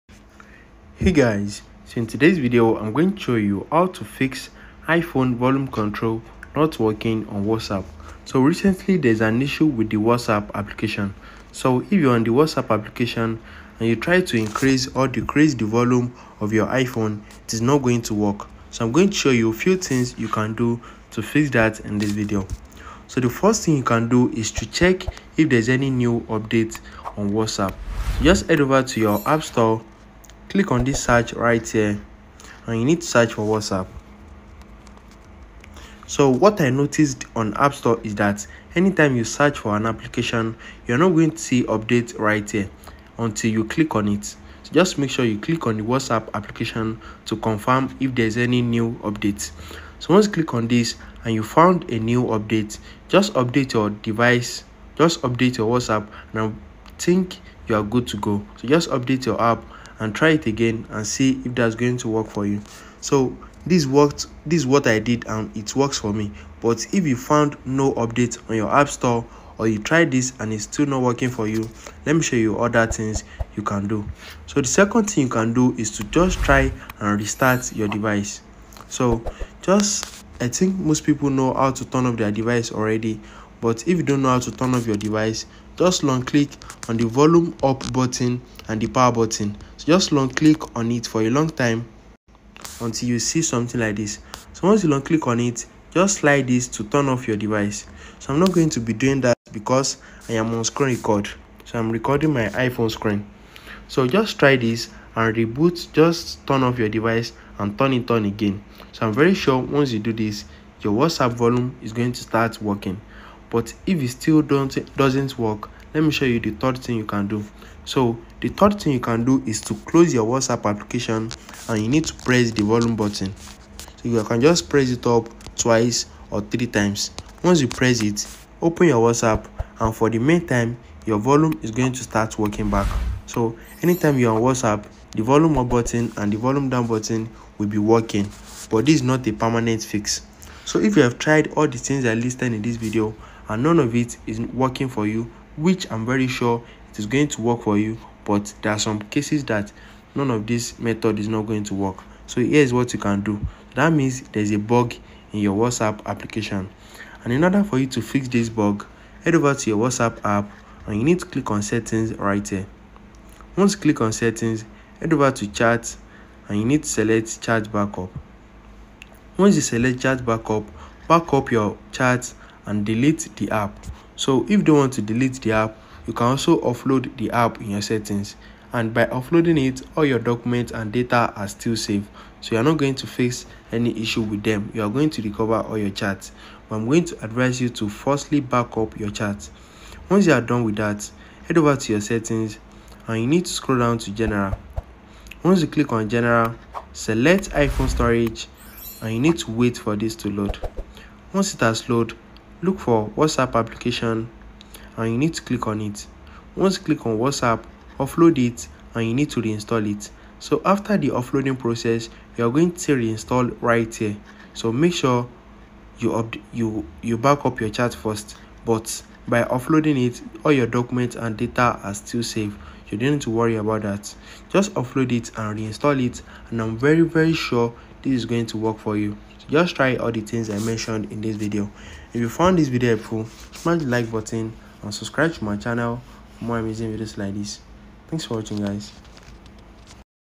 How to Fix iPhone Volume Control Not Working on WhatsApp
Tired of silent WhatsApp? Learn how to fix iPhone volume control issues on WhatsApp and never miss a video/audio again. Troubleshoot with our step-by-step guide. Watch now for clear, crisp audio! 🔊📱